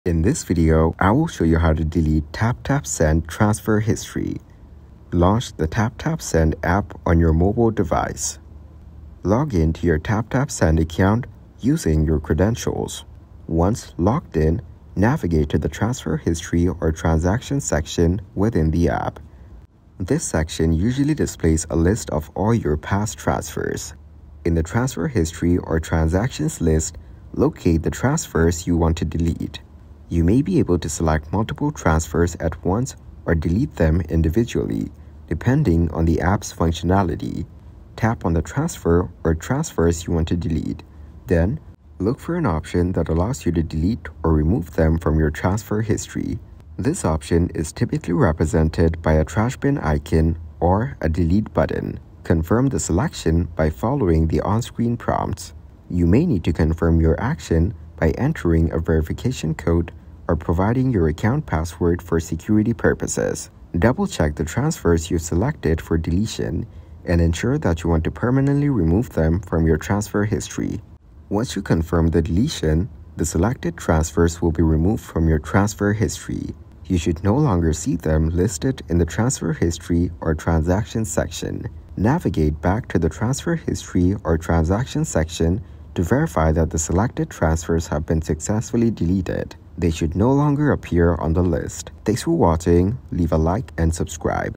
HOW TO DELETE TAPTAP SEND TRANSFER HISTORY (FULL GUIDE)
In this video, I will guide you How to Delete Taptap Send Transfer History